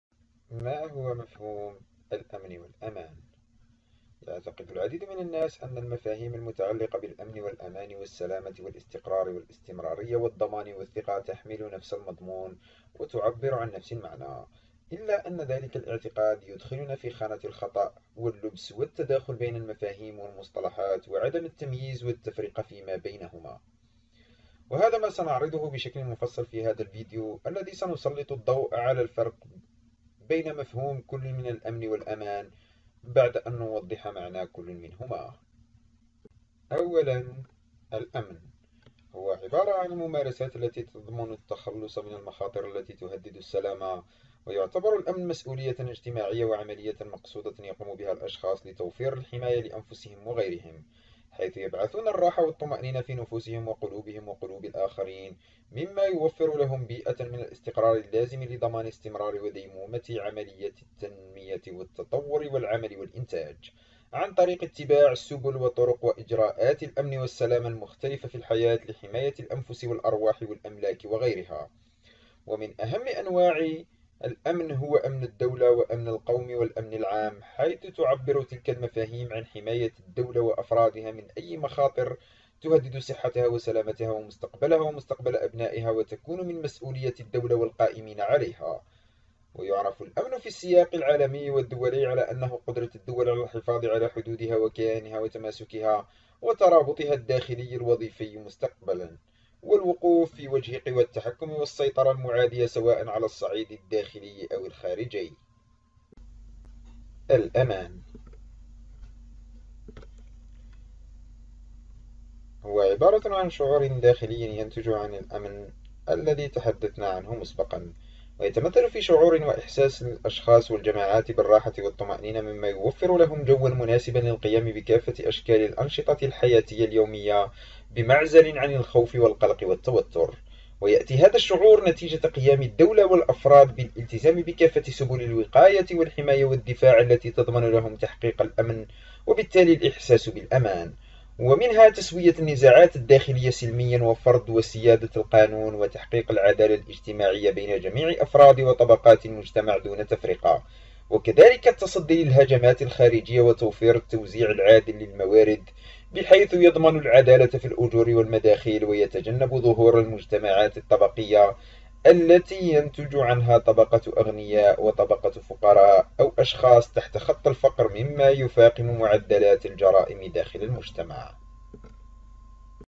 ما هو الفرق بين الأمن والأمان ؟
يعتقد العديد من الناس أنّ المفاهيم المتعلّقة بالأمن، والأمان، والسلامة، والاستقرار، والاستمرارية، والضمان، والثقة تحمل نفس المضمون وتعبر عن نفس المعنى، إلّا أنّ ذلك الاعتقاد يدخلنا في خانة الخطأ واللبس والتداخل بين المفاهيم والمصطلحات وعدم التمييز والتفرقة فيما بينها، وهذا ما سنعرضه بشكل مفصل في هذا المقال الذي سنسلط الضوء فيه على الفرق بين مفهوم كل من الأمن والأمان، بعد أن نوضح معنى كل منهم.

Many people believe that the concepts related to security, safety, safety, stability, continuity, guarantee, and trust carry the same content and express the same meaning, but that belief introduces us to error, confusion, overlap between concepts and terms, non-discrimination and discrimination among them, and this is what we will present In detail in this article, in which we will highlight the difference between the concept of security and safety, after we clarify the meaning of each of them.